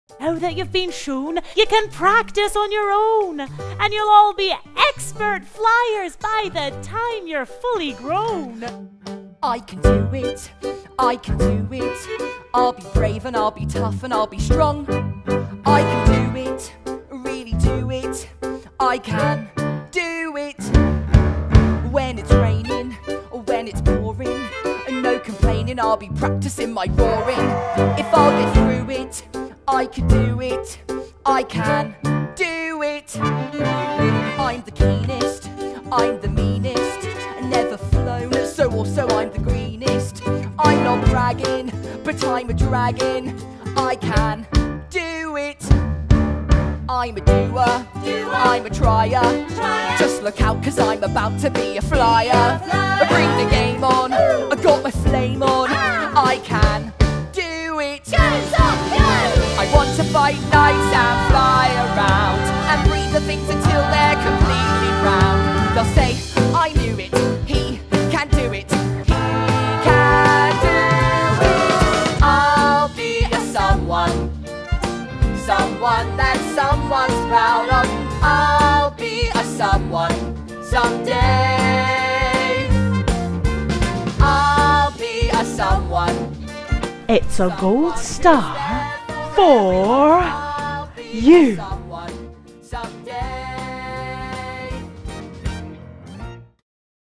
Zog - Your Trailer
Performing at Royal & Derngate, Northampton from Thu 29 June.

Based on the book by Julia Donaldson and Axel Scheffler

Large in size and keen in nature, Zog is eager to win a golden star at Madam Dragon’s school, where dragons learn all the things that dragons need to know.  Zog tries so very hard, perhaps too hard, and he bumps, burns and roars his way through years one, two and three.  Luckily, the plucky Princess Pearl patches him up ready to face his biggest challenge yet…a duel with knight, Sir Gadabout the Great!

The smash-hit stage adaptation from Freckle Productions (the team behind Zog and the Flying Doctors and Stick Man) is back and promises to be bigger, brighter and more roar-some than ever before! With lively songs from Joe Stilgoe and directed by Emma Kilbey, Julia Donaldson and Axel Scheffler’s much-loved Zog is a magical production for all ages.